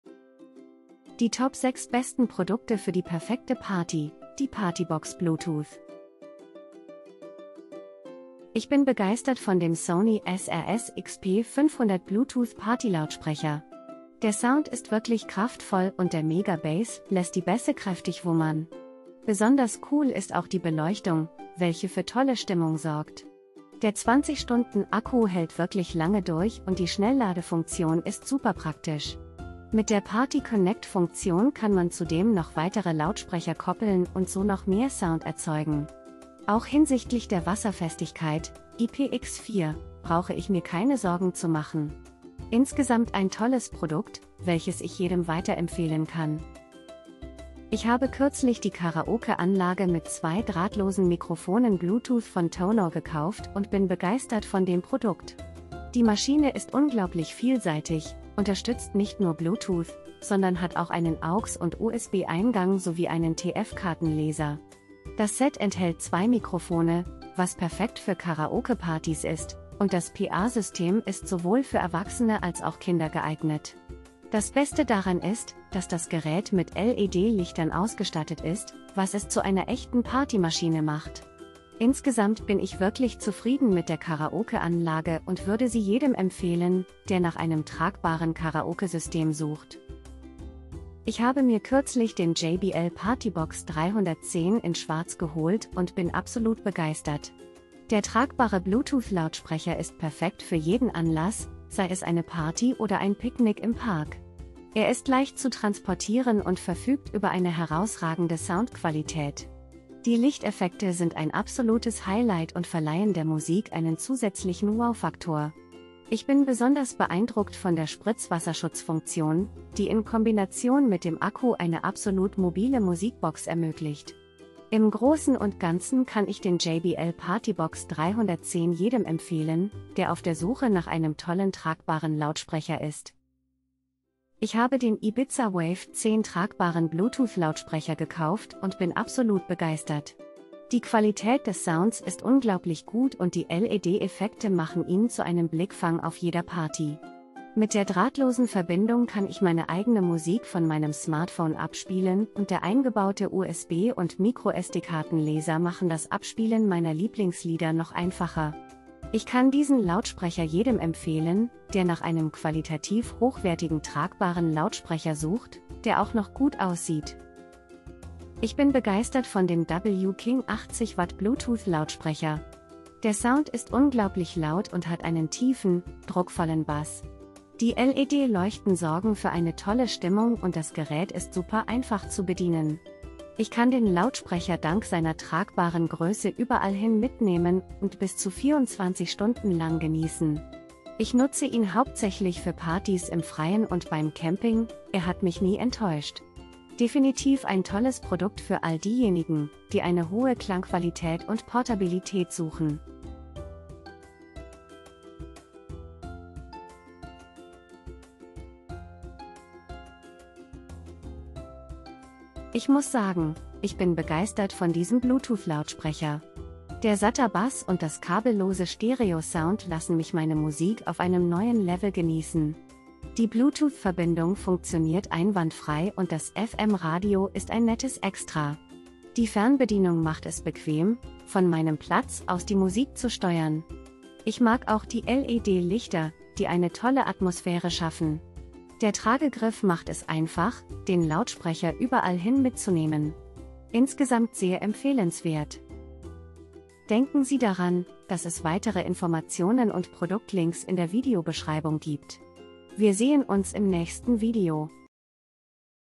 【PARTYBOX BLUETOOTH】Die Top 6 besten Produkte für die perfekte Party: Die PartyBox Bluetooth
✋  Hier sind die top 6 besten produkte für die perfekte party: die partybox bluetooth auf dem Markt.

🔽🔽🔽 KAUFEN-LINKS PARTYBOX BLUETOOTH 🔽🔽🔽

1) Sony SRS-XP500 Bluetooth Partylautsprecher mit kraftvollem Sound, Beleuchtung und 20h Akku (IPX4, MEGA BASS, Schnellladefunktion,Party Connect) Schwarz

2) Karaoke Anlage mit 2 drahtlosen Mikrofonen Bluetooth, TONOR Karaoke Maschine Lautsprecher mit LED Licht Partymaschine mit Wireless Microphone Musikbox mit AUX/USB/TF PA-System für Erwachsene/Kinder K6

3) JBL PartyBox 310 in Schwarz – Trag- und rollbarer Bluetooth Party-Lautsprecher mit Lichteffekten – Spritzwassergeschützte, mobile Musikbox mit Akku

4) Ibiza WAVE10 800W/2x10 Lautsprecher mit Bluetooth, USB und microSD - LED-Effekte und drahtlose Verbindung TWS - Schwarz

5) Bluetooth Lautsprecher, W-KING 80W Tragbarer Kabelloser Lautsprecher Box Musikbox, Laut mit Druckvollem Bass, Gewaltigem 105db Sound, Gemischtfarbigen LED-Leuchten, USB-Wiedergabe,24 Stunden Spielzeit

6) Bluetooth Lautsprecher, Tragbare Bluetooth Box mit Subwoofer, kabellos Stereo Sound satter Bass Party Lautsprecher unterstützt FM Radio Fernbedienung LED Lichter (Black)

* Zeitindikatoren (Timestamps) *

00:00 - Intro
00:07 - 1) Sony SRS-XP500 Bluetooth Partylautsprecher mit kraftvollem...
00:46 - 2) Karaoke Anlage mit 2 drahtlosen Mikrofonen Bluetooth, TONOR...
01:29 - 3) JBL PartyBox 310 in Schwarz – Trag- und...
02:13 - 4) Ibiza WAVE10 800W/2x10 Lautsprecher mit Bluetooth, USB und...
02:47 - 5) Bluetooth Lautsprecher, W-KING 80W Tragbarer Kabelloser...
03:31 - 6) Bluetooth Lautsprecher, Tragbare Bluetooth Box mit...
04:05 - Outro